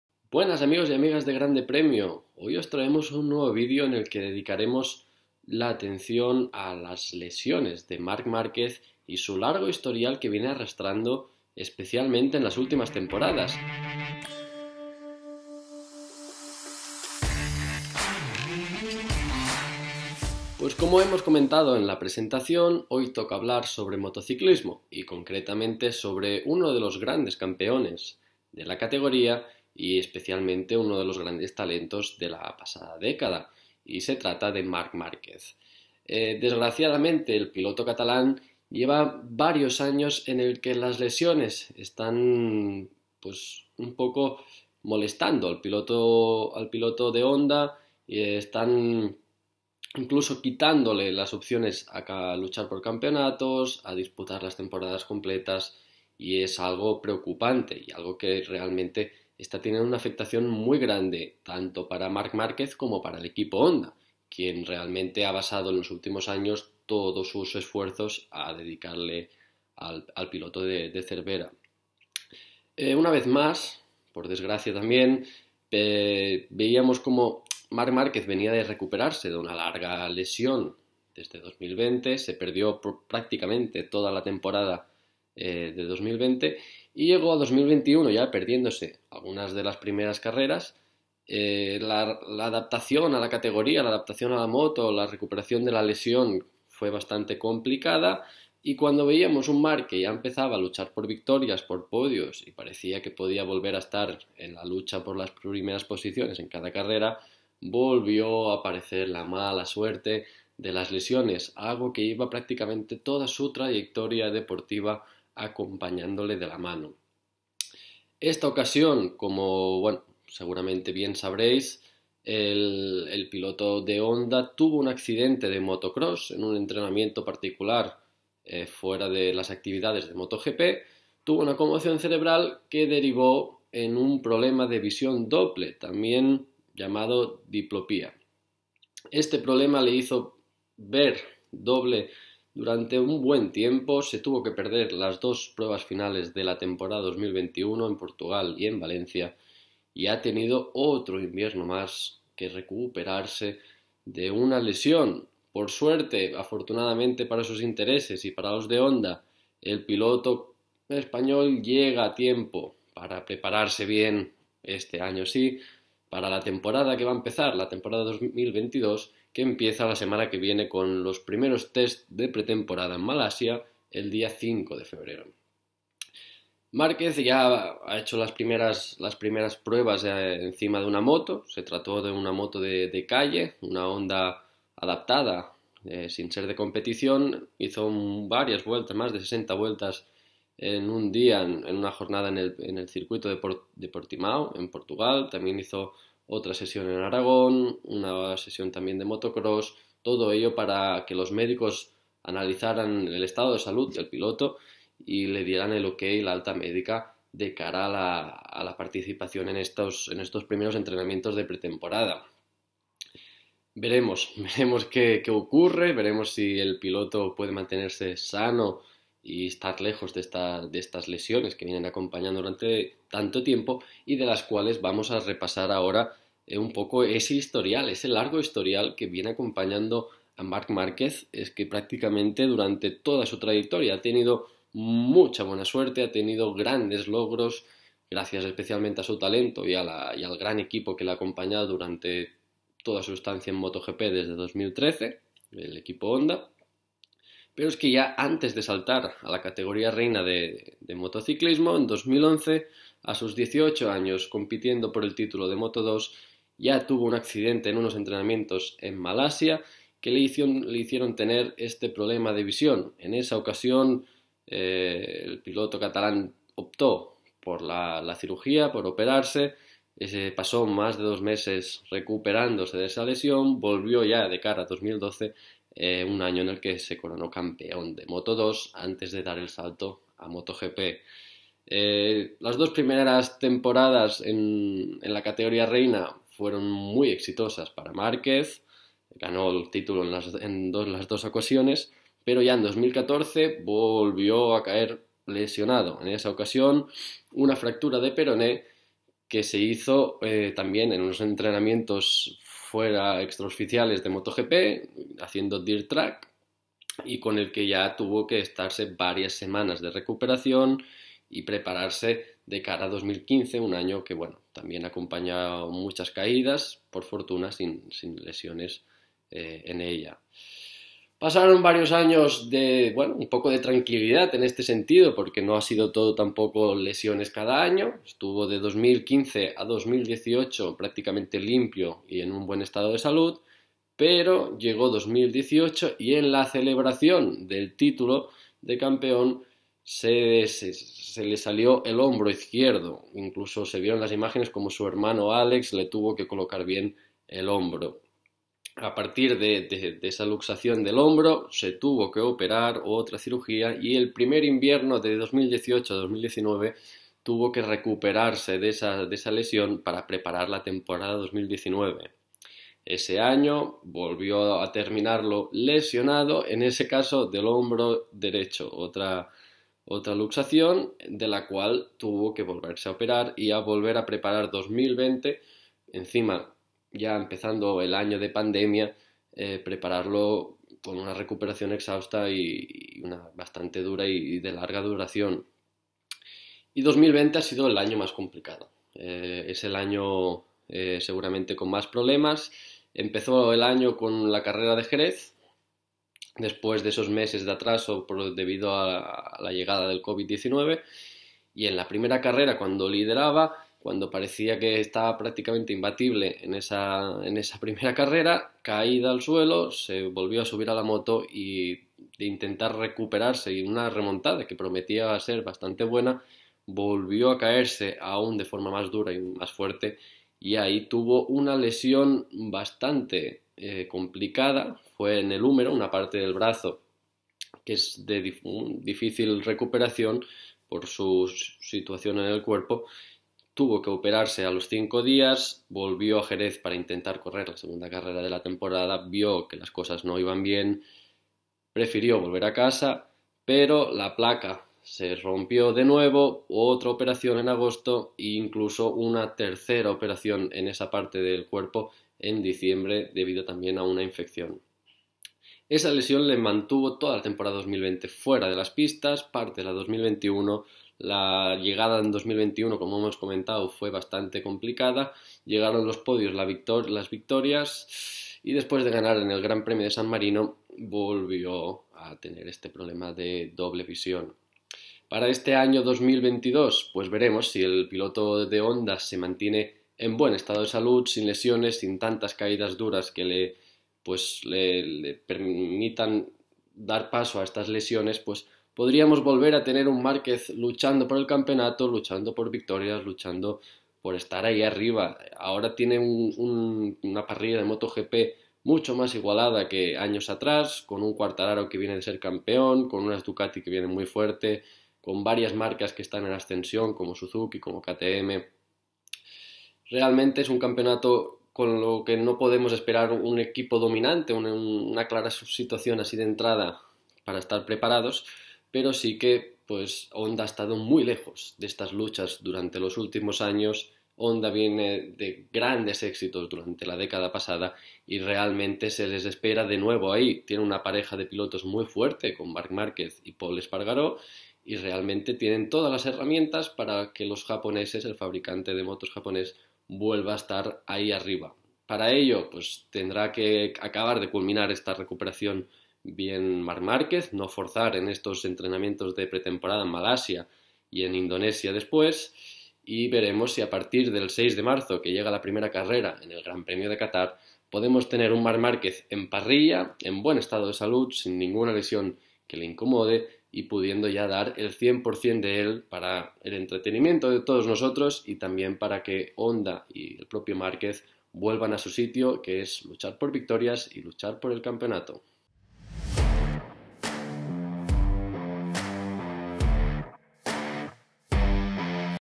MARC MÁRQUEZ Y EL LARGO HISTORIAL DE LESIONES QUE ARRASTRA EN MOTOGP
Marc Márquez regresó recientemente a las pistas tras recuperarse de la enésima lesión. Esta última fue debido a un caso de doble visión, que le apartó de la moto para las últimas dos carreras de la temporada 2021.

Eric Calduch repasa el largo historial de lesiones de Márquez desde 2011, cuando ya vivió un complicado caso de doble visión. Además, analiza las expectativas de regreso a MotoGP, donde volverá a competir este próximo mes de febrero para las pruebas de pretemporada en Malasia e Indonesia.